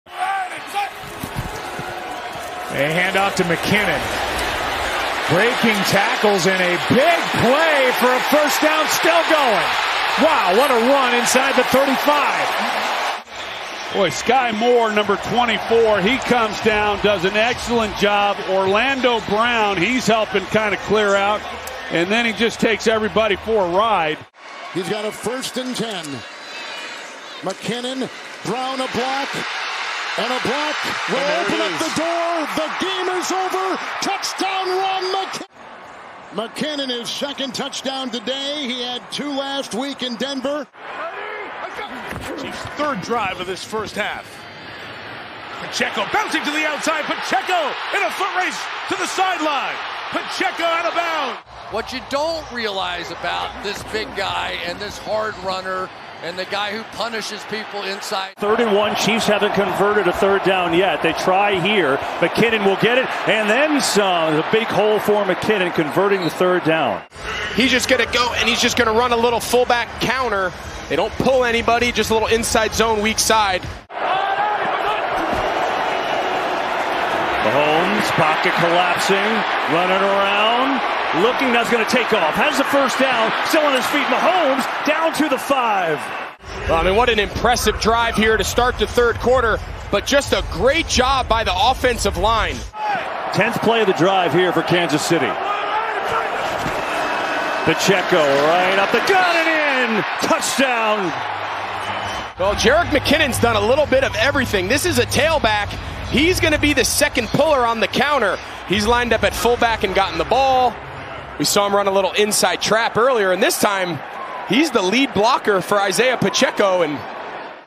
Orlando Brown Jr: The Newest Addition to the Bengal's Offensive Line
Orlando Brown Jr's best career plays.